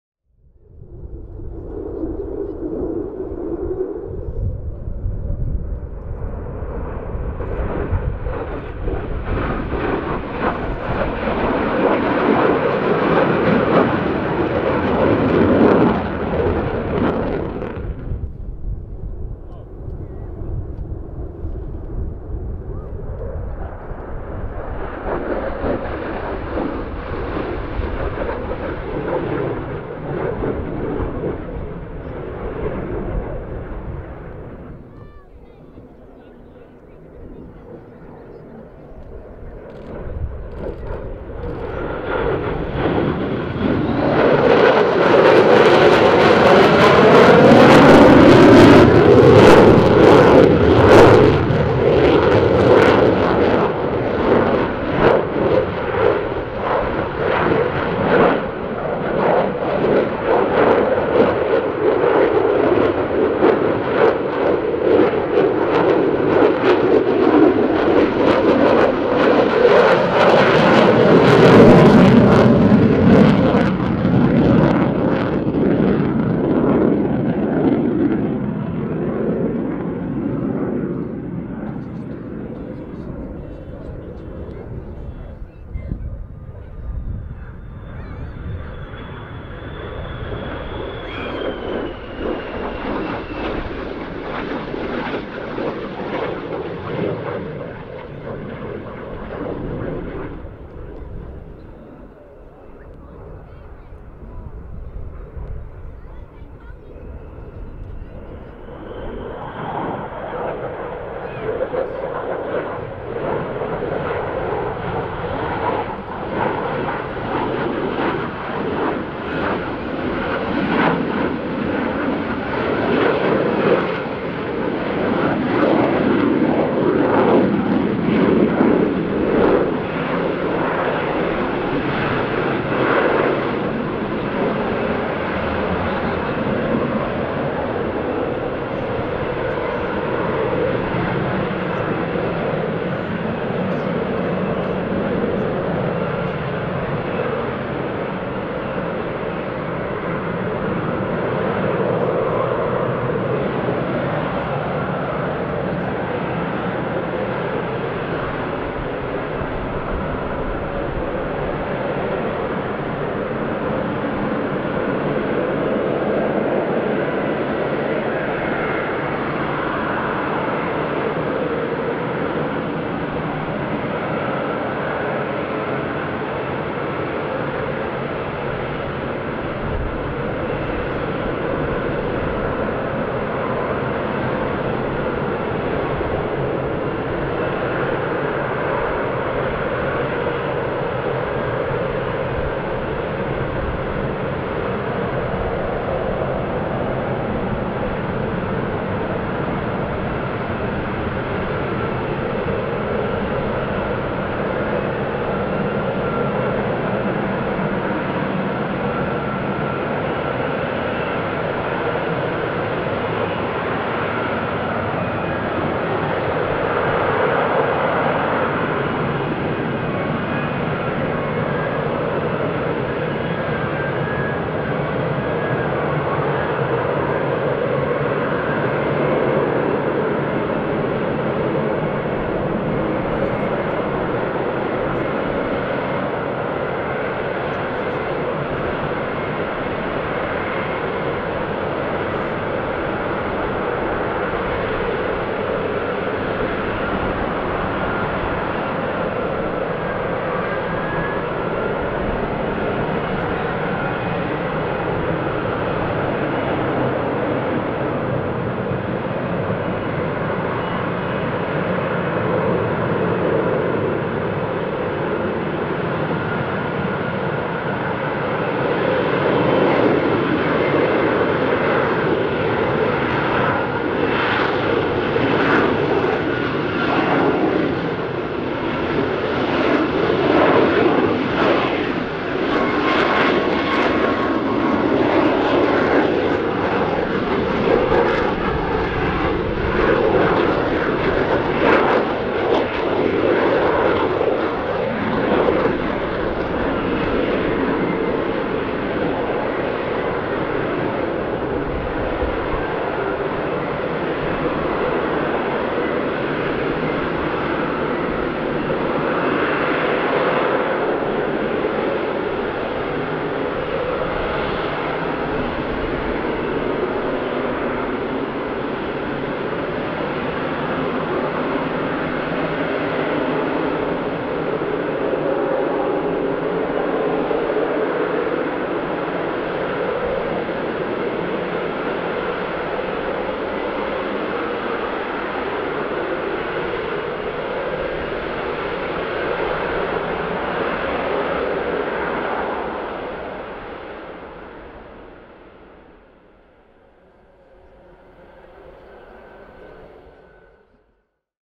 Overhead F-35 Lightning II display ⚡️⚡️⚡️ - RIAT 2016
The fist two passes of this display woke us all up!

This particular aircraft is an F-35B (ZM137) and is now stationed at RAF Marham. This jet came over from the USA for the 2016 show and was finally delivered to Marham in the summer of 2019.

This video was shot from Totterdown Farm on July 10th 2016.